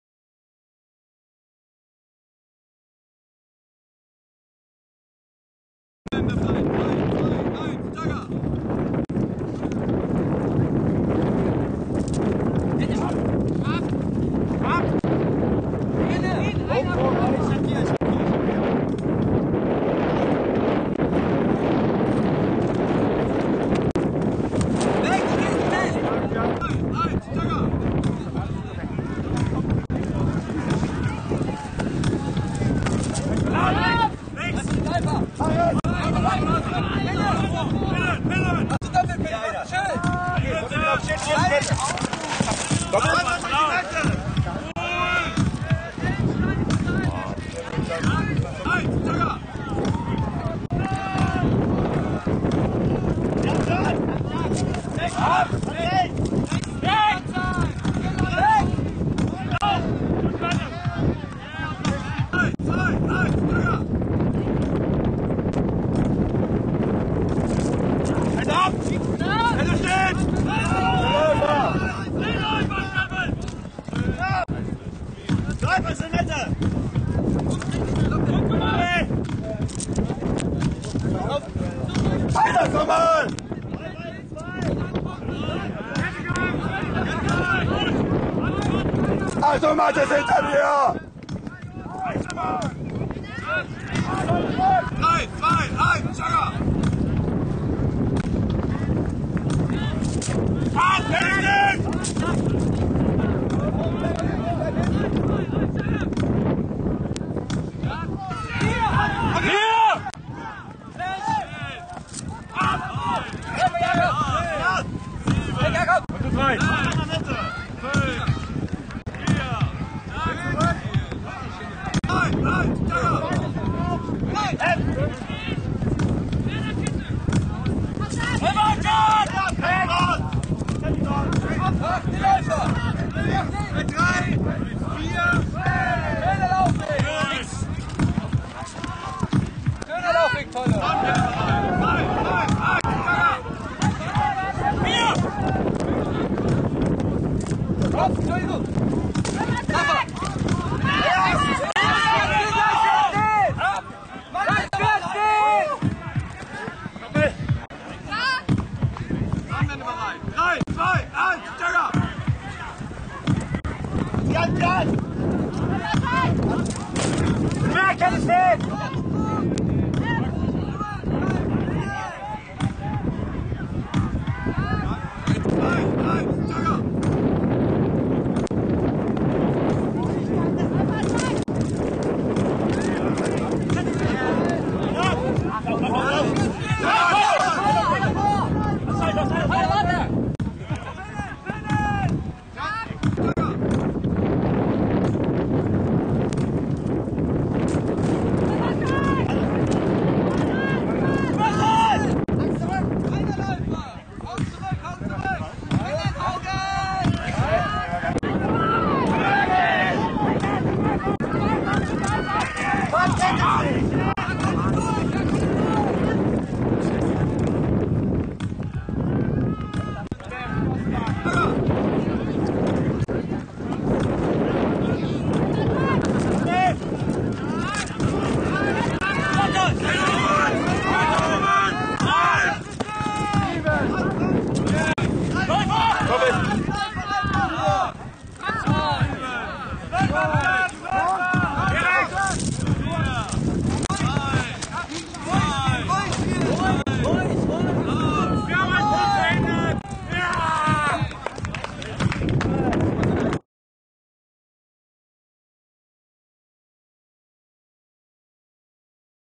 2013 DM Zonenkinder vs Sonnenwende
komplettes Spiel - 16. Deutsche Juggermeisterschaft - Juggerturnier - Achtelfinale - Zonenkinder (Jena) vs. Sonnenwende (Berlin) - Date: 22. September 2013 - Ort: Tempelhofer Freiheit - Veranstalter: Jugger e.V. - Ranking: 1. Murcia Jugger Selection - 2. Rigor Mortis 2 (Berlin) - 3. Rigor Mortis (Berlin) - 4. Skull! (Berlin);

Movie by Susanne Serwe & Lester Balz